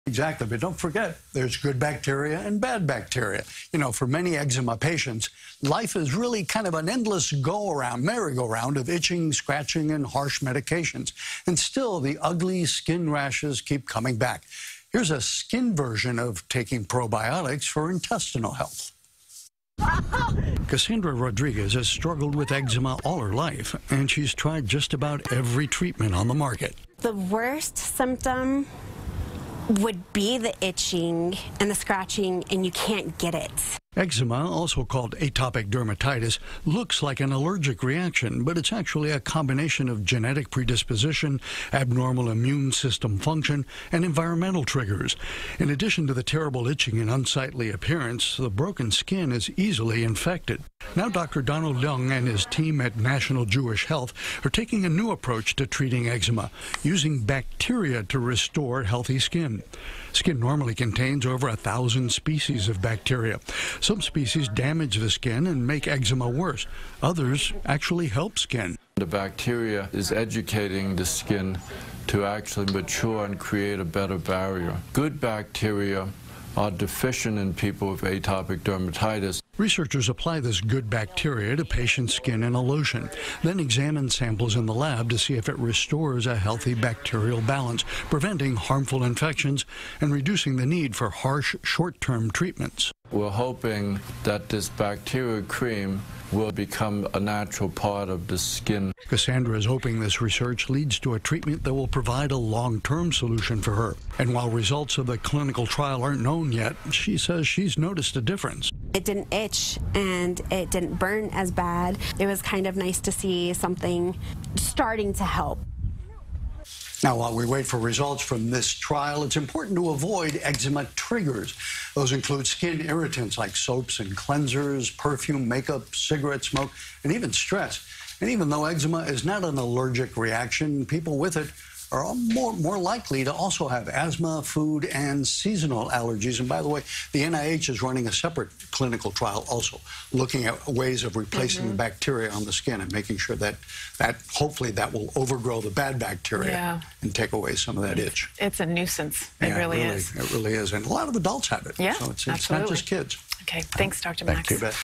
Bacteria Treatment For Eczema?
There’s some potentially good news for sufferers of the persistent, itchy rash called eczema. As CBS2’s Dr. Max Gomez reported, there’s now a clinical trial using bacteria to treat this.